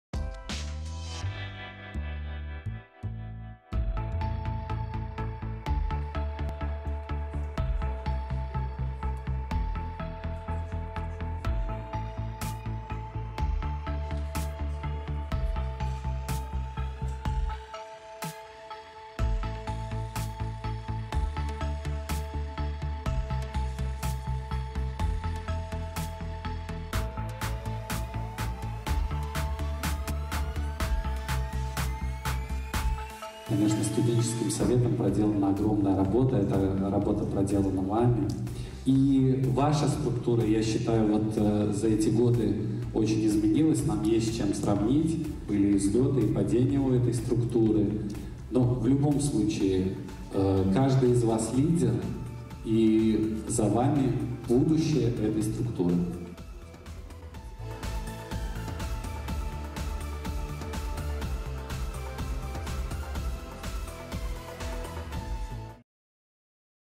ОТЧЕТНО-ВЫБОРНАЯ КОНФЕРЕНЦИЯ СТУДЕНЧЕСКОГО СОВЕТА УНИВЕРСИТЕТА
30 ноября 2021 года в БГПУ прошла отчетно-выборная конференция студенческого совета университета. На конференции прозвучал отчет действующего председателя студенческого совета БГПУ Павла Лапатинского о работе за период с апреля 2018 года по ноябрь 2021 года.